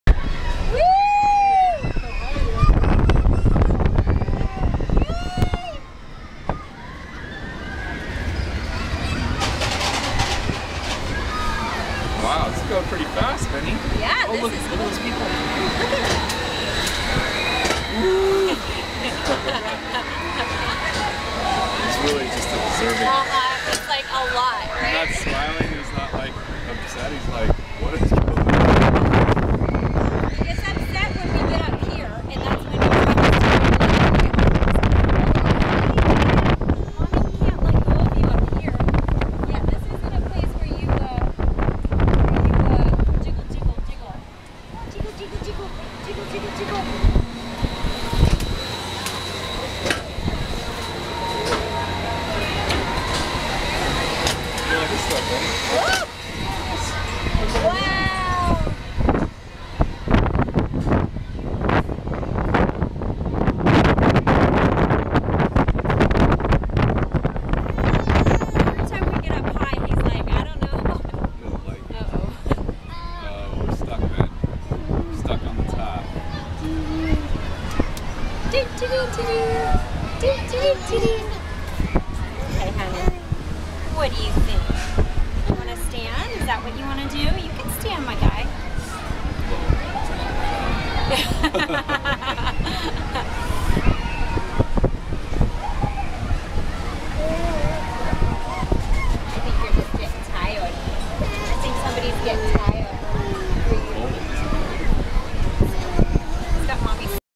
Ben's first ride on the Ferris Wheel in Santa Monica 2015-06-07
Sunday afternoon in Santa Monica on the Pier